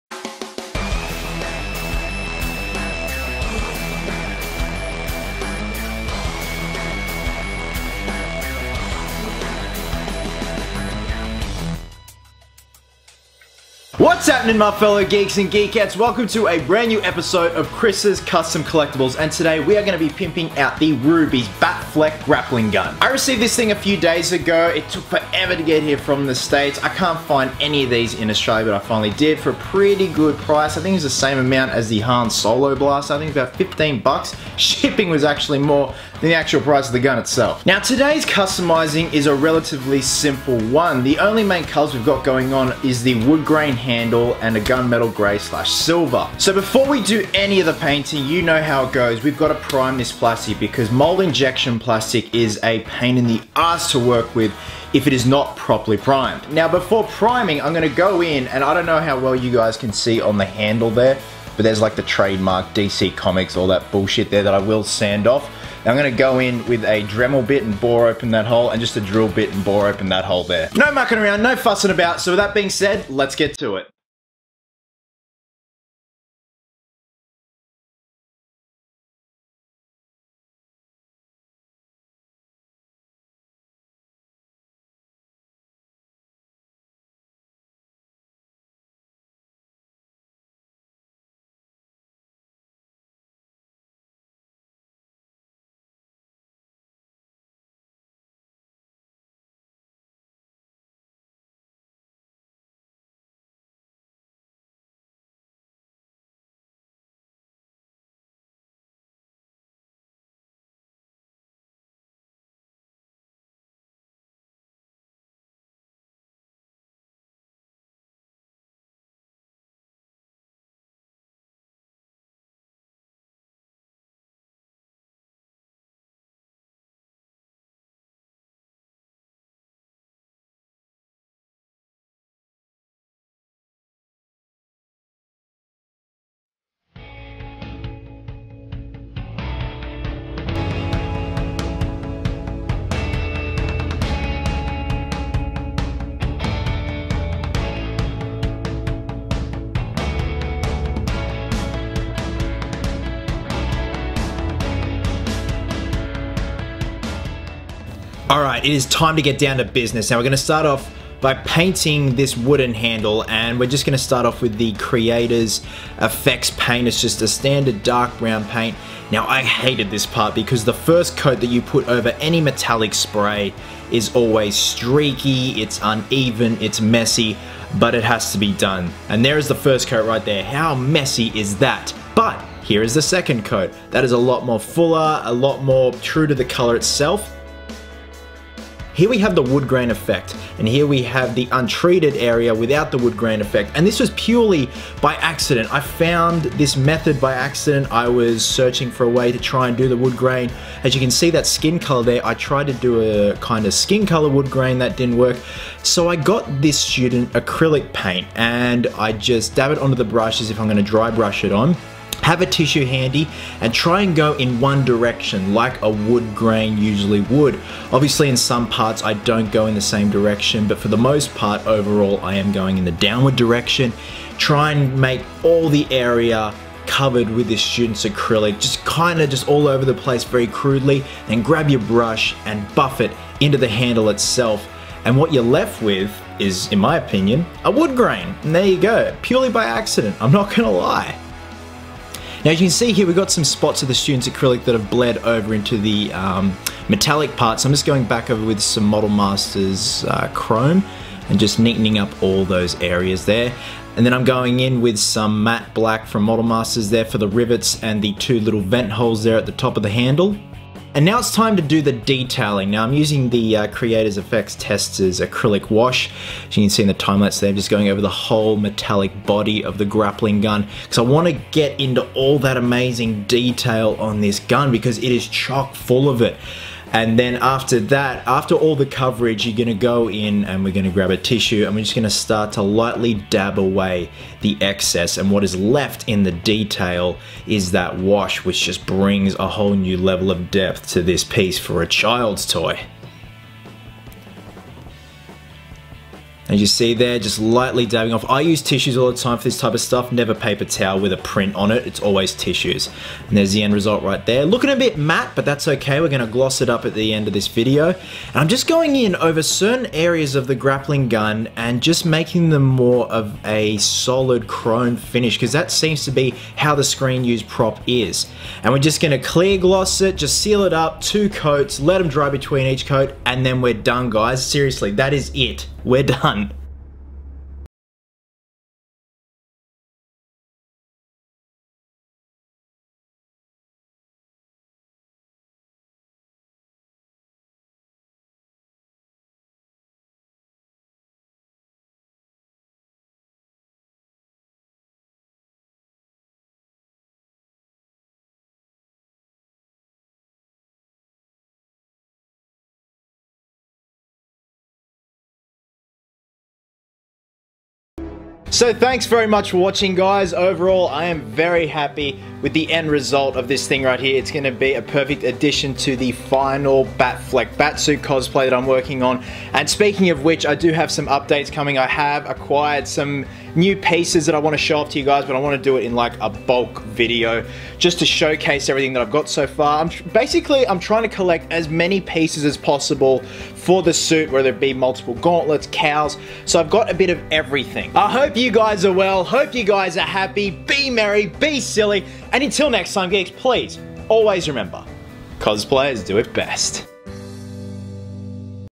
Rubies Dawn of Justice Batman Grappling Gun Makeover- Quick Custom Collectable!
It's Custom Collectable time, Geeks! This week I'll be pimping out the Rubies Batfleck Grappling gun. Its a simple yet effective process :)